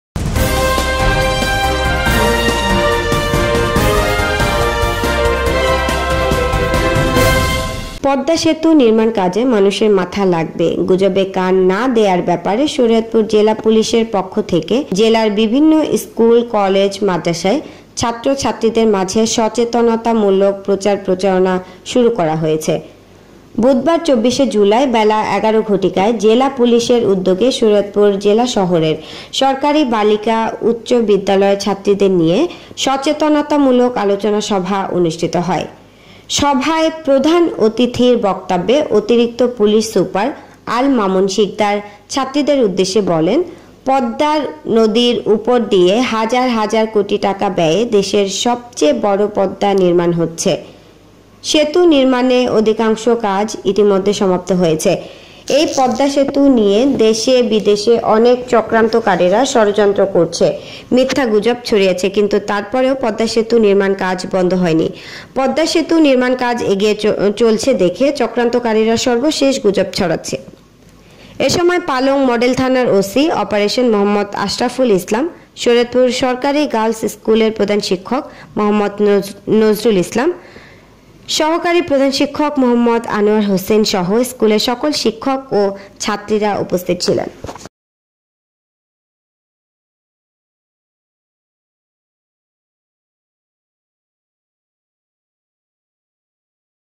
পদ্মা সেতুতে মানুষের মাথা !
পদ্মা সেতুতে মানুষের মাথা, গুজবে শরীয়তপুর জেলা পুলিশের পক্ষ থেকে জেলার বিভিন্ন স্কুল-কলেজ মাদ্রাসায় ছাত্র-ছাত্রীদের মাঝে সচেতনতামূলক প্রচারণা শুরু করা হয়েছে।